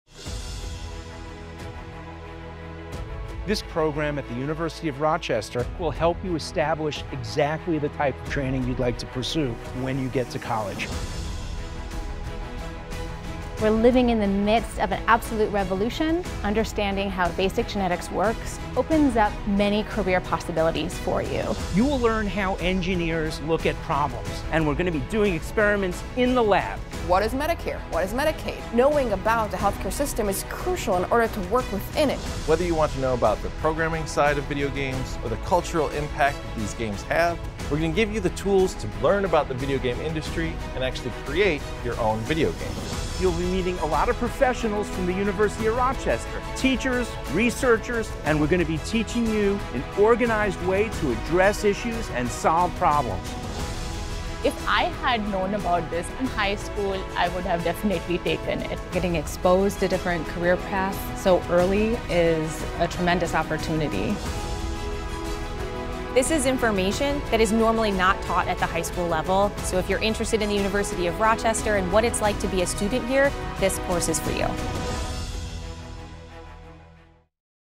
Rochester Program Trailer | Rochester Pre-College Online Program
University of Rochester’s online program for high school students is designed to help you identify exactly the type of degree and inspiring careers you’d like to pursue.

Let’s start with biomedical engineering. Biomedical engineering is the application of engineering tools and technology to address problems in medicine and biology. You’ll identify the stages of engineering design; delve into medical innovation; and learn how to measure, collect and display data.

As for pre-college science, our medicine course unlocks the mysteries of human genetics which has made the field of personalized medicine possible. This course is for anyone who has a passion to study gene sequencing and understand what it is about your genetic make-up that makes you unique.

Orthopedic medicine delves into the surgical and non-surgical specialties of this popular field. And, U.S. healthcare reveals the enormous changes that have taken place since the introduction of the Affordable Care Act.

Finally, for the digitally-minded, we offer video game design, a course that shares how video games are built, and shows you how to customize a pre-built demo and turn it into your own 2D platforming game.

The Rochester Pre-College Online Program is designed for high school students 13 and older. Our college-prep curriculum features dynamic video lessons and interactive tools. You’ll enjoy mentor support as you complete courses that will prepare you for a college major and possible future careers.